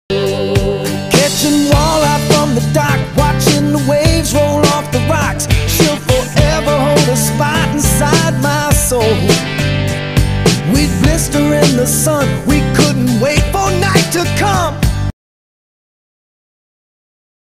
Fish a DOCK around the Clock for BIG BASS 🎣
Don't just fish a DOCK one time, fish come and go around the Clock ⌚ so always fish them at least twice.